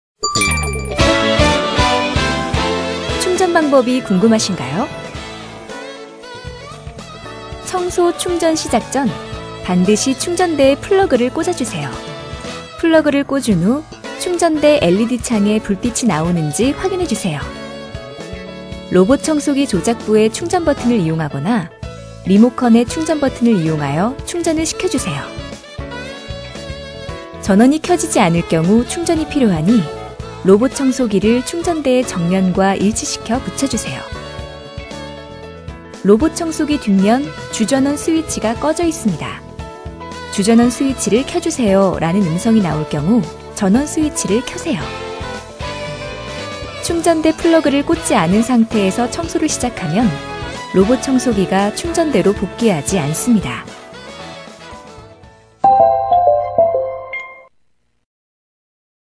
LG 코드제로 R9 - 충전 방법
충전대의 플러그를 반드시 꽂아 주세요.
이후 충전대 LED에 불이 들어오는지 확인하세요.
로봇청소기의 조작부나 리모컨을 사용해 충전을 시작해 주세요.
주전원 스위치가 꺼져 있음을 알리는 음성이 나올 경우 전원 스위치를 켜 주세요.